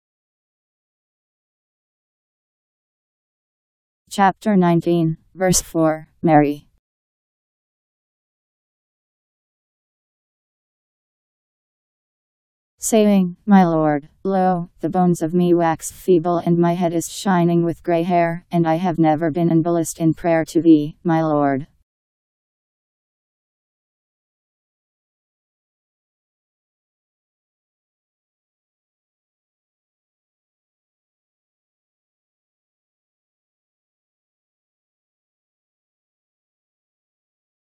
Quran Mary 19:4
Video shows Quran Mary Chapter 19 Verse 4: Saying: My Lord! Lo! the bones of me wax feeble and my head is shining with grey hair, and I have never been unblest in prayer to Thee, my Lord. Powered by MaryTTS. Holy Qur'an 19:4 Surah : Ayah